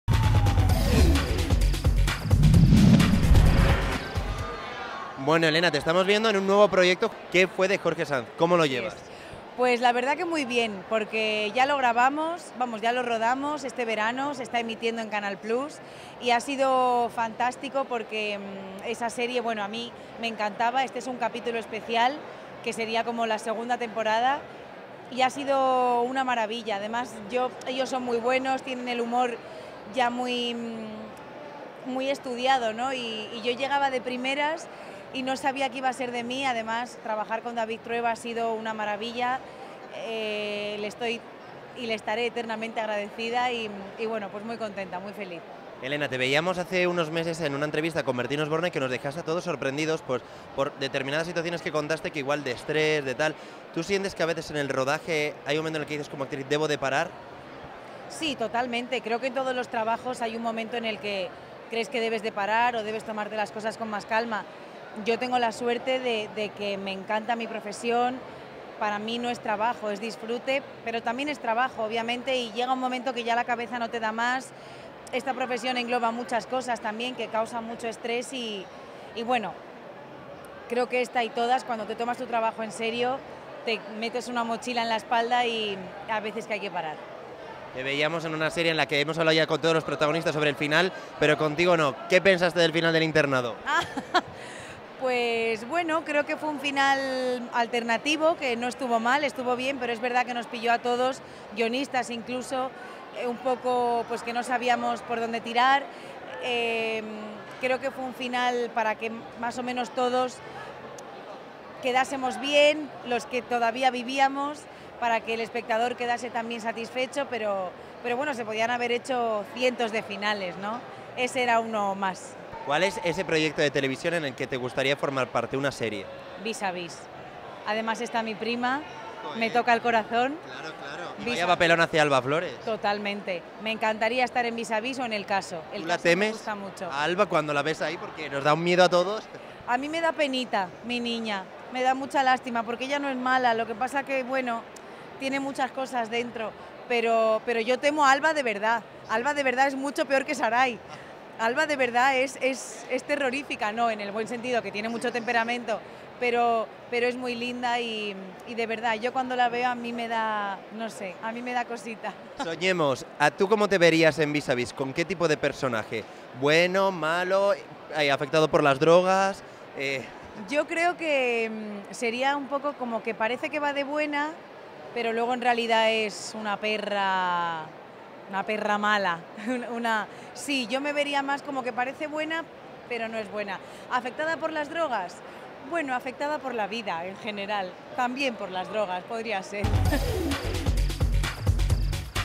Elena Furiase se sincera y opina sobre el final de 'El internado'
¿La actriz se ha llegado a sentir presionada y ha tenido que alejarse por ello de la televisión? ¿En qué serie querría participar?